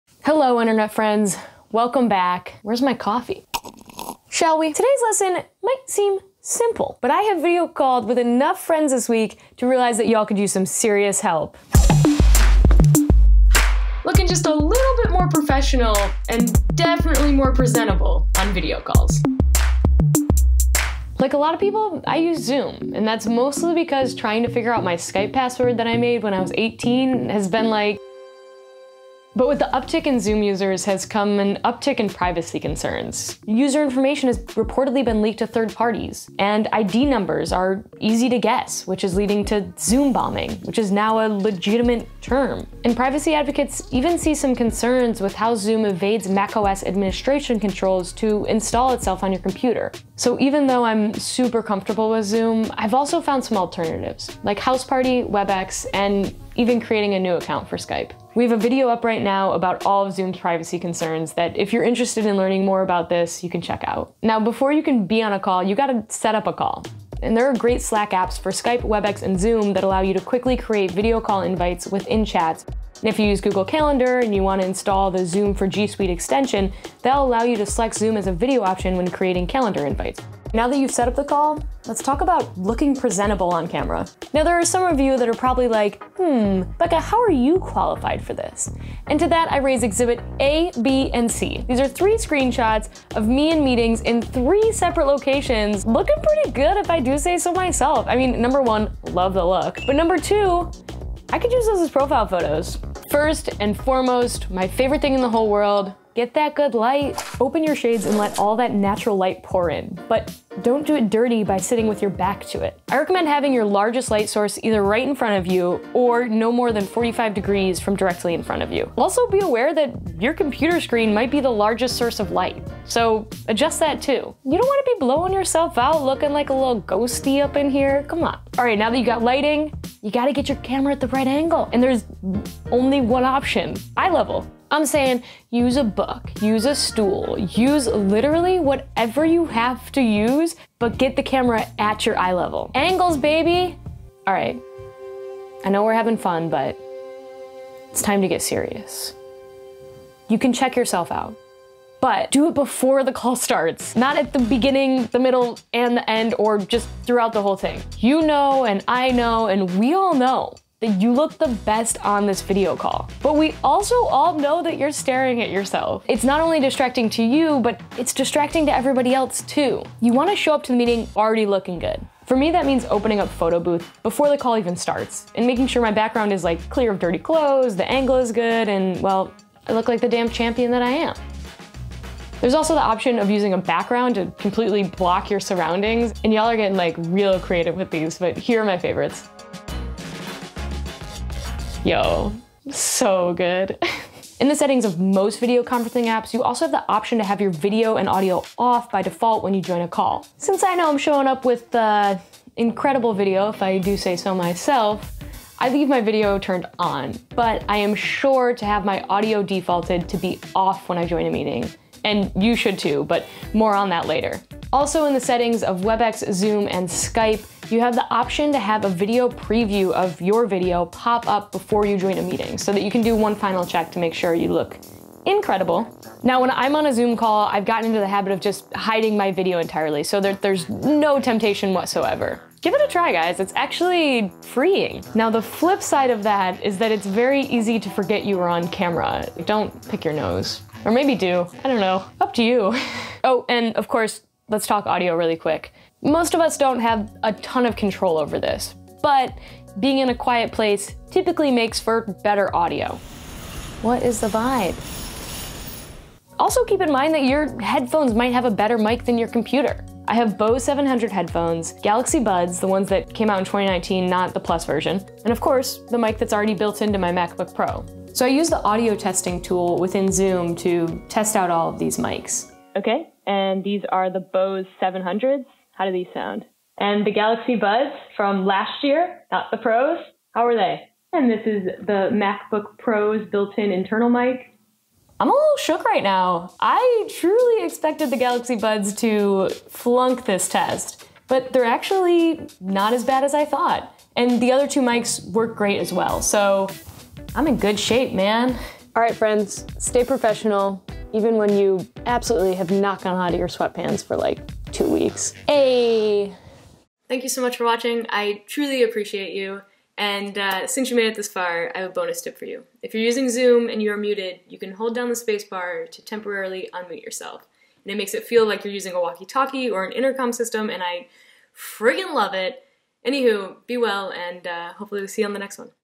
How to improve your video calls in 5 minutes
From virtual weddings to work meetings, we all suddenly have to be on camera in our homes, and as a Verge video director and host, Becca Farsace is no stranger to having to be camera ready at all times and in imperfect spaces. Here are some tips and tricks to becoming the video call MVP you were meant to be.